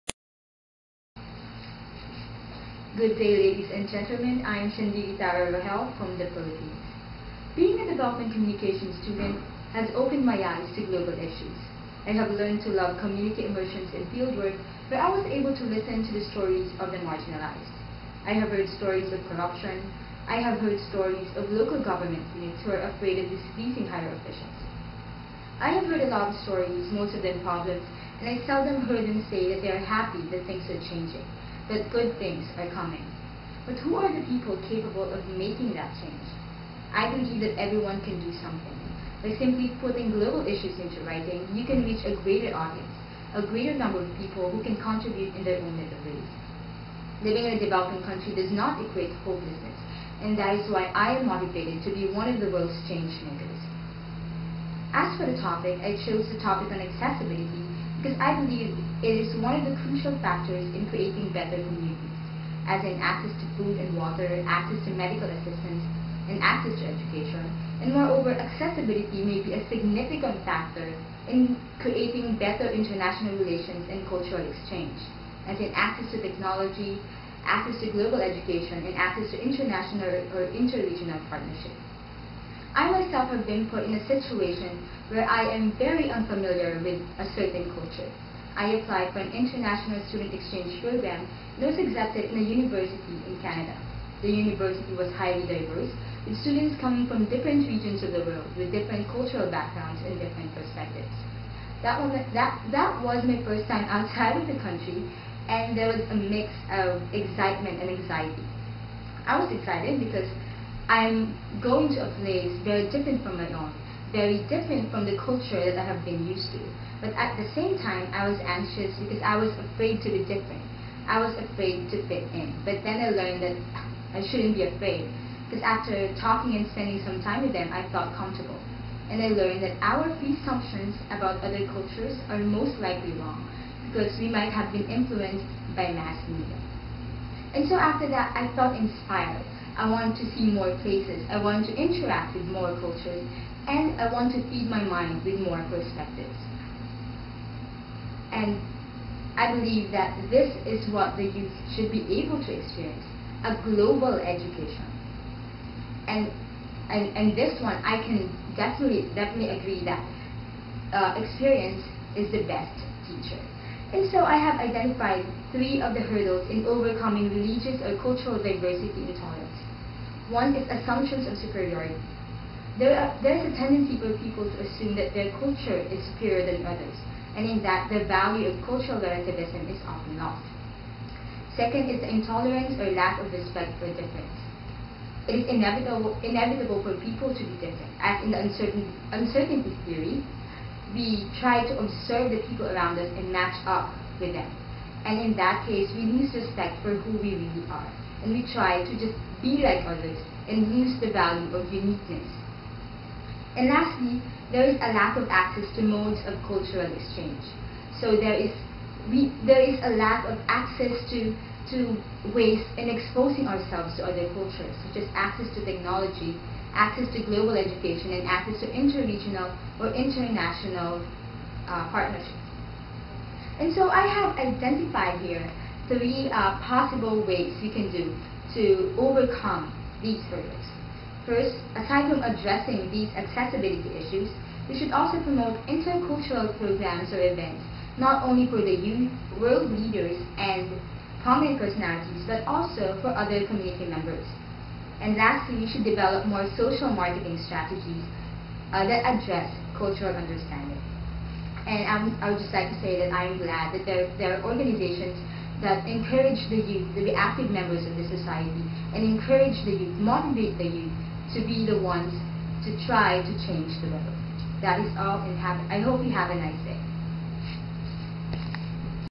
Essay Competition Winner_International Category_Chndyli Tara G Rogel.flv
Unity in Diversity Forum
Kerala, India 6-8 February 2011
Essay Competition Winner, International Category